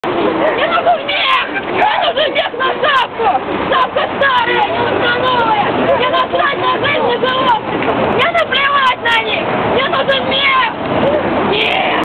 Action against baby-seals murder, Moscow Jan 27. #3
Animal rights activists from aliance for animal rights made a demo against murder of baby seals in North of Russia.